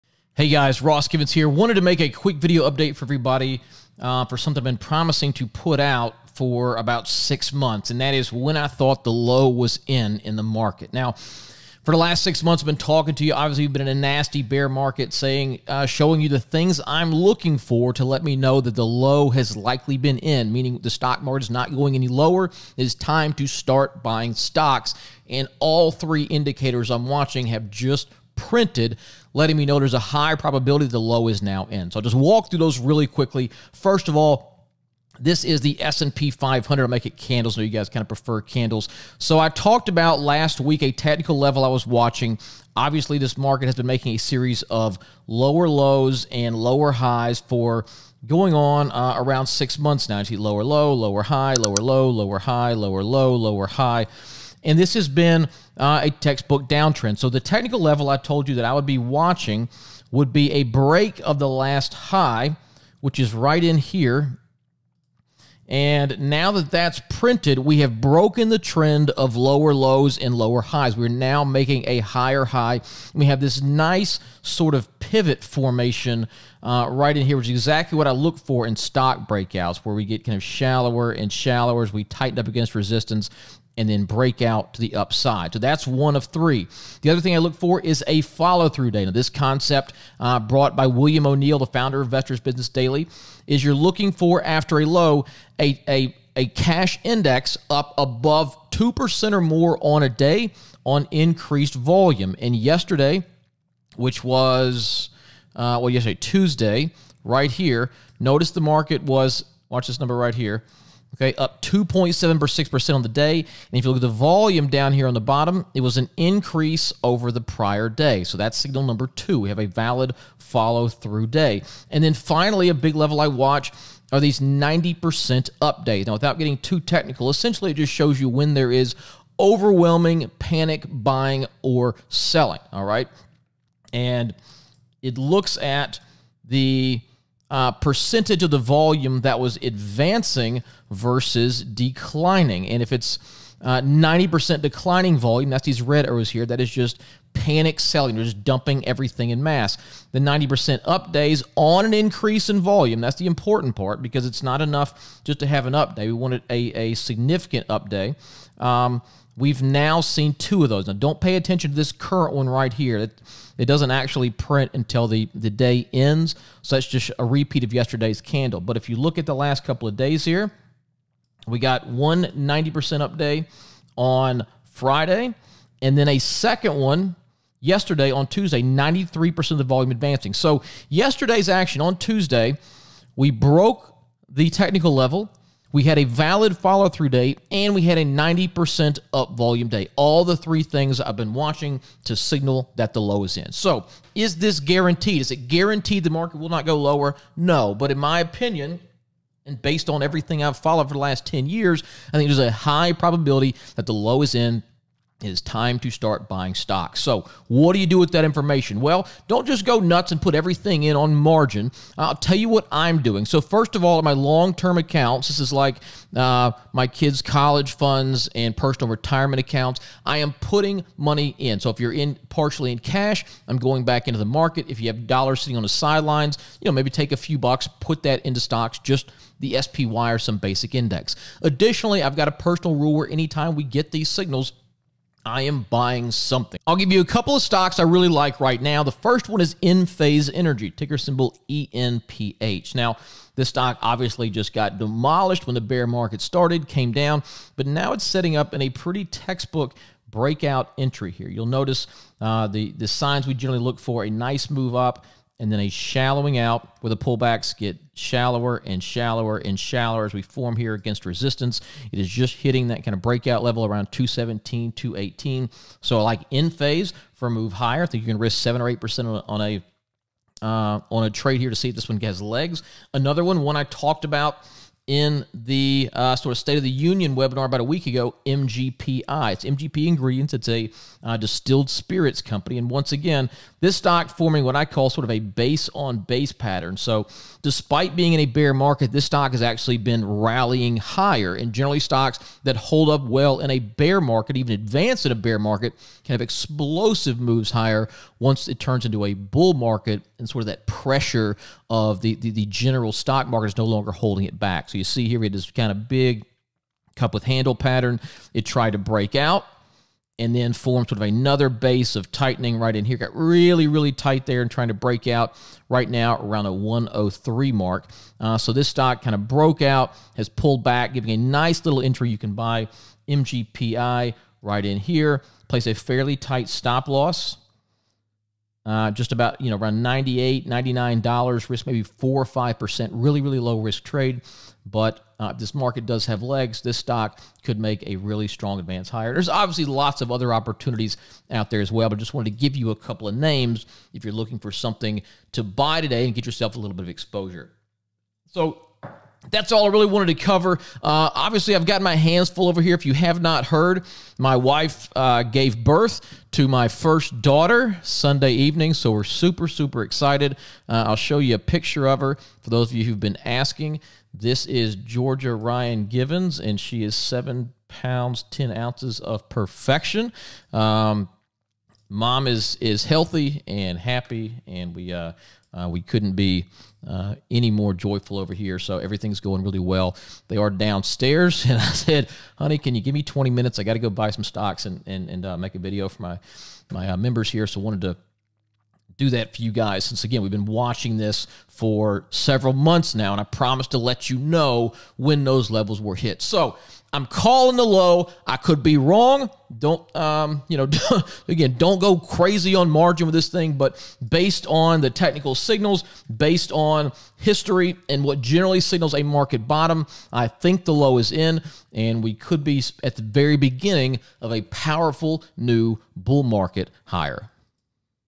I'm calling the low in the market
For the last 6 months, I have been sharing with members the 3 signals I was watching to signal that the market has bottomed. All 3 have now triggered, so there is a high likelihood the low is in. In this video, I break down each of those signals and discuss my plans. I did not cover them in this video, but 2 other internal signals have also printed bullish. We just had the 3rd day in a row with more stock making new highs than lows, and stock above their 50-day moving average exceeded 50%.